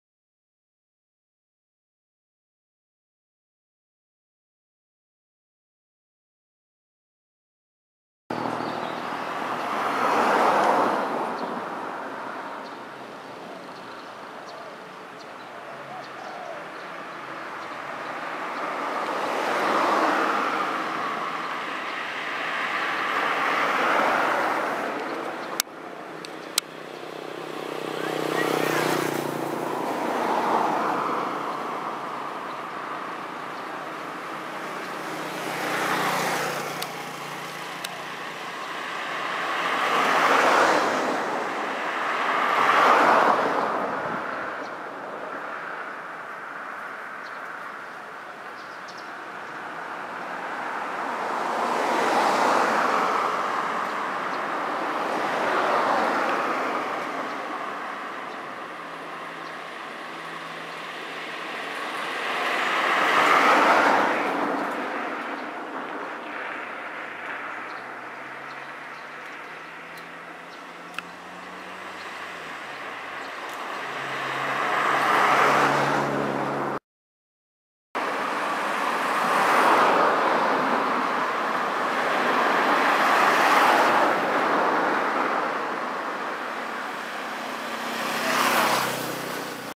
Terkukur Sg.Ara@Bt. Gajah 15 Feb15 bh 2
@BtGajah, Perak. this wild bird spar very fast and after many rounds, (not shown) final caught but loosen from racik..and fly away